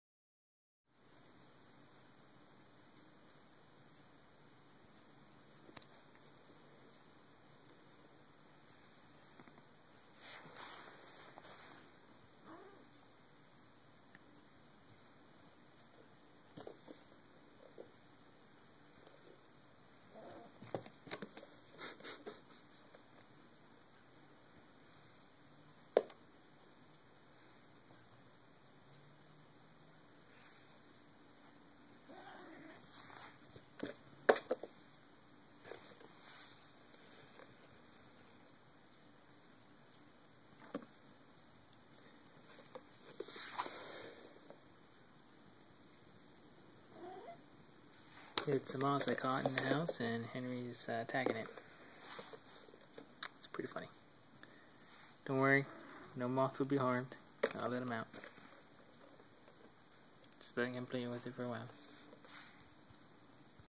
Moth caught, Cat interested
caught a moth in the house. cat wanted a piece of it.